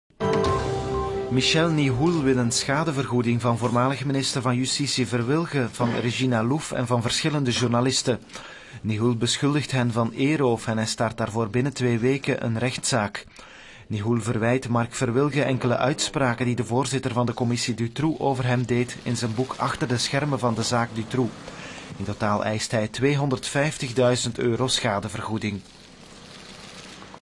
Michel Nihoul eist schadevergoeding van Marc Verwilghen
Michel Nihoul, die ooit beschouwd werd als de spin in het web van de zaak-Dutroux, vraagt op 19 november voor de burgerlijke rechtbank van Brugge 250.000 euro schadevergoeding van Marc Verwilghen. Nihoul verwijt Verwilghen uitspraken die de huidige minister van Economie over hem deed in zijn boek Achter de schermen van de zaak-Dutroux.